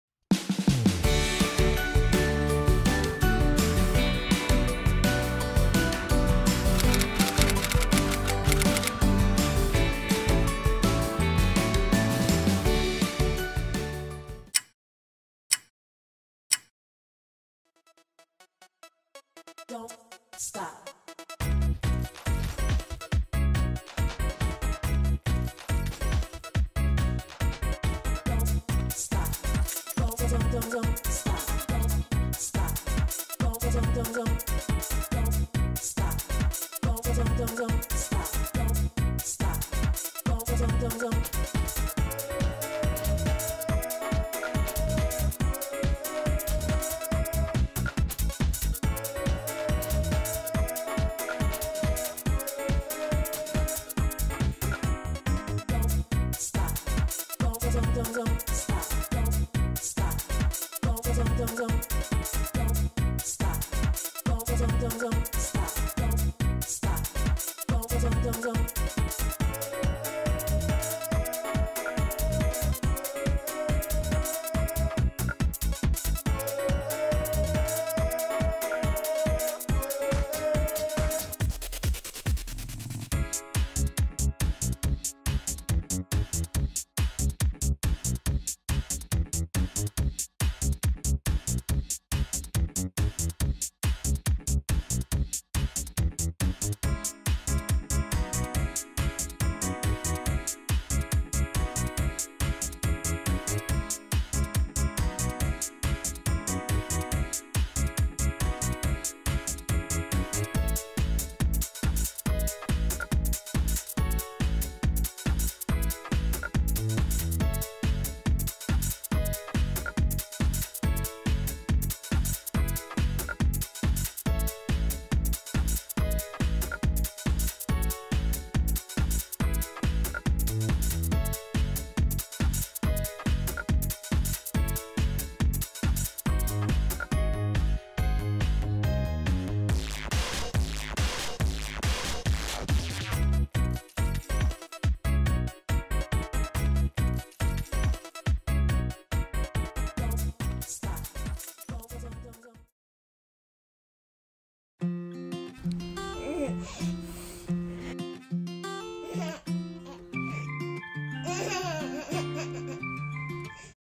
【#066●8倍速】スプーンぶんぶんテーブルばんばん顔真っ赤でうなる…苦戦。Hana swings the spoon about and bangs the table..violent...
『0歳8ヶ月の赤ちゃんハナがひたすら美味しそうに離乳食を食べる動画』
“8-month-old baby girl Hana happily eats her solid food:)"

♡Hana's items♡

ライスシリアル

ベビーチェア baby table&chair